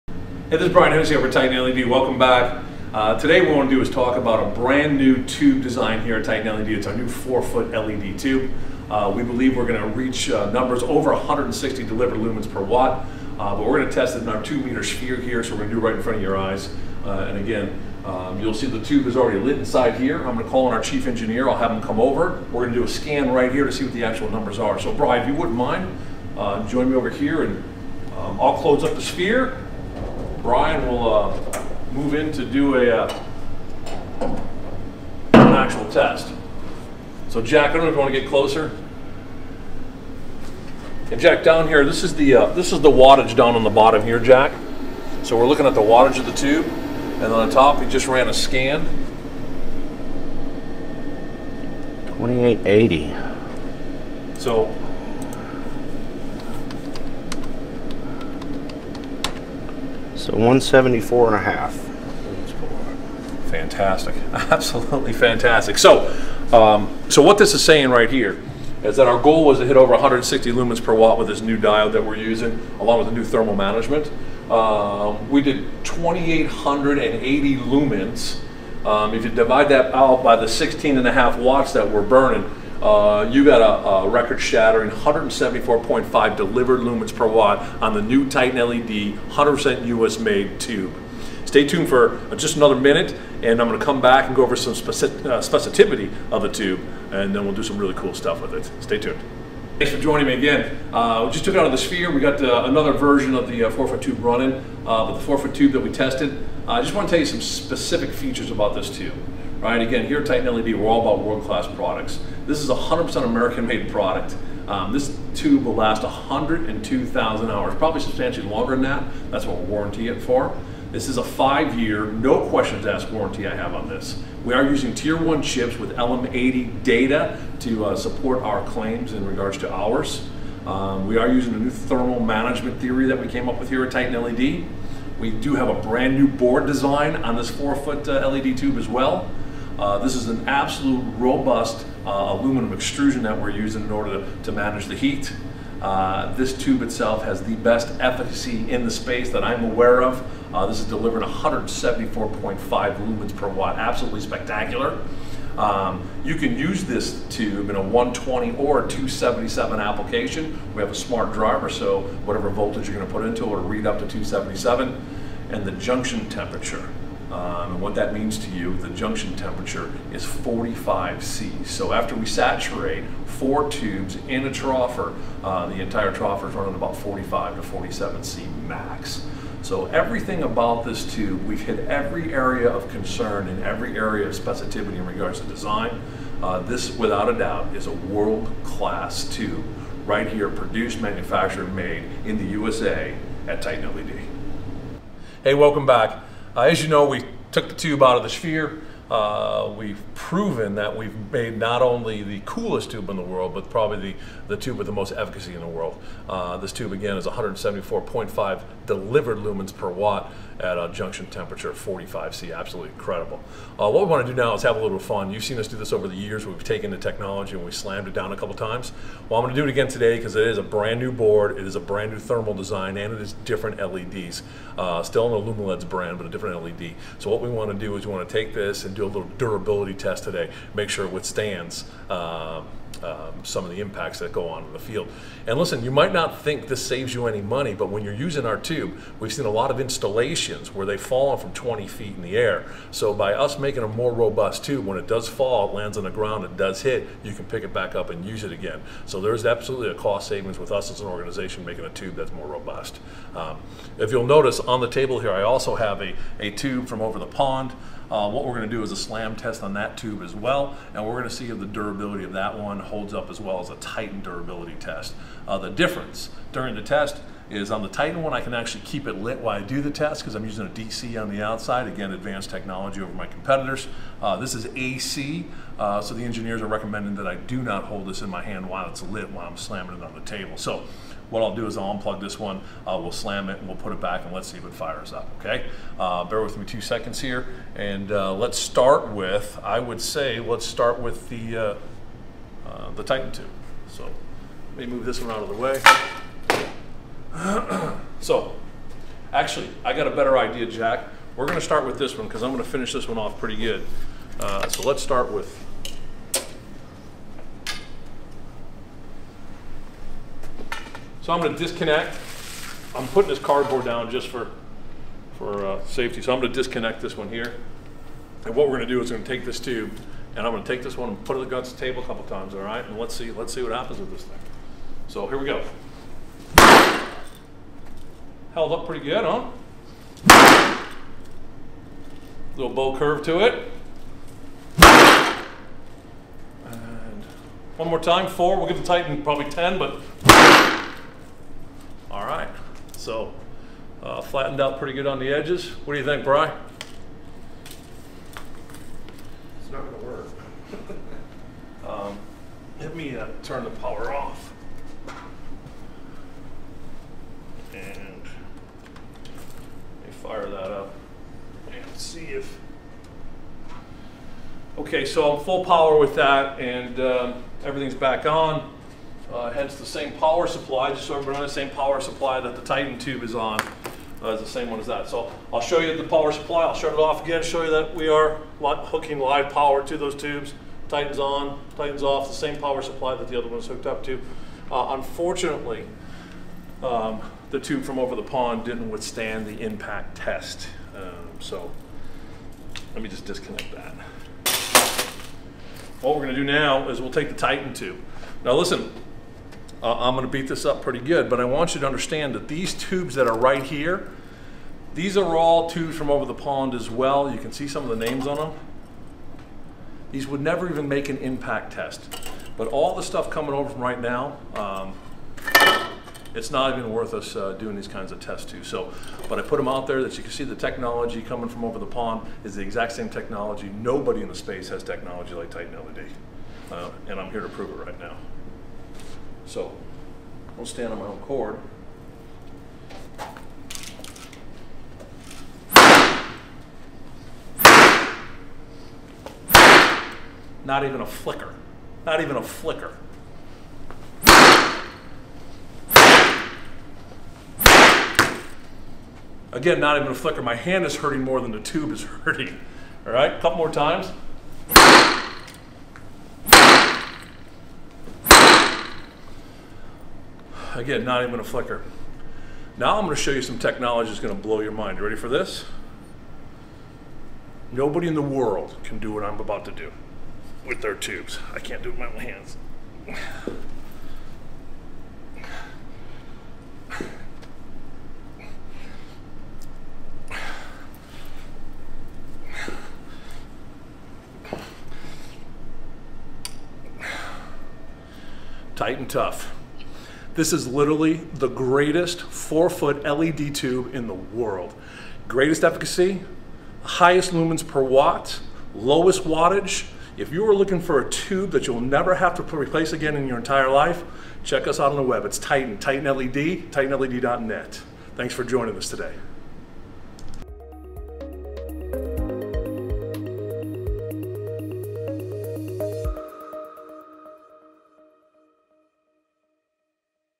American Made Titan LED Tubes vs Over The Pond Tubes in 4K
We tested our new 4 foot LED tube in our 2 meter sphere and the results were incredible. Our new American made LED tubes produce 174.5 delivered lumens per watt! After running the scan we tested the durability of our Titan LED tube and an over the pond tube by slamming both of them against a table. The tube from over the pond did not withstand the impact test while the Titan tube kept running!

Titan LED Tube features:

- 100% American made product
- 5 Year Warranty
- Tier 1 Chips
- Thermal Management
- New Board Design
- Best Efficacy
- Delivers 174.5 Lumens Per Watt
- Compatible in 120 and 277 Applications
- Junction Temperatures is 45 C